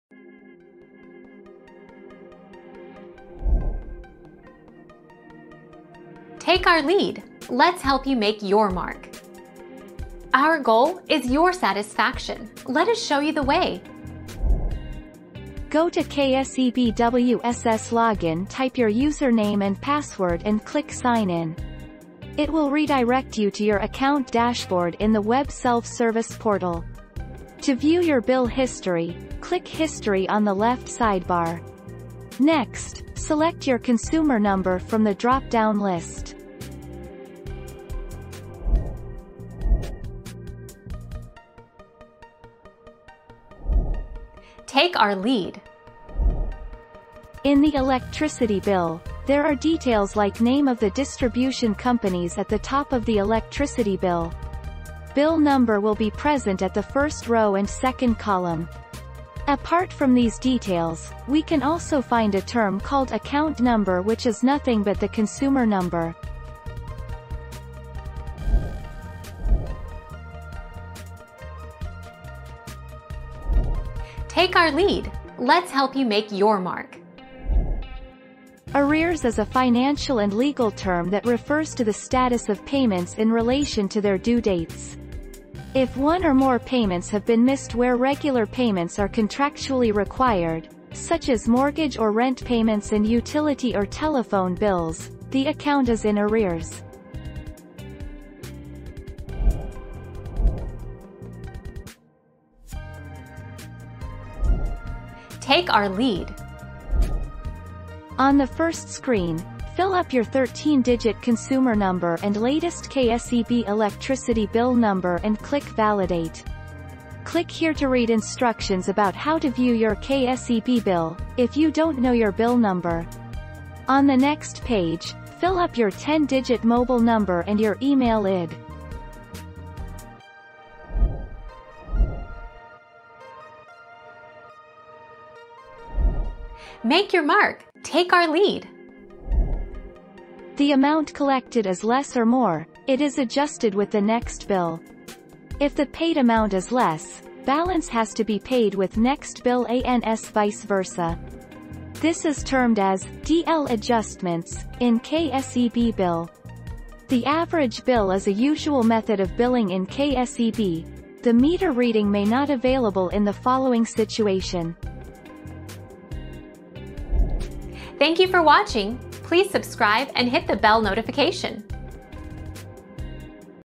How can I get my old Kseb bill?
00:00 - How can I get my old Kseb bill?
00:40 - Where is consumer number in electricity bill?
01:10 - What does arrear mean in electricity bill?
01:40 - How can I add my mobile number in Kseb?
02:12 - What is DL adjustment in Kseb bill?

Laura S. Harris (2021, January 11.) How can I get my old Kseb bill?
    AskAbout.video/articles/How-can-I-get-my-old-Kseb-bill-205634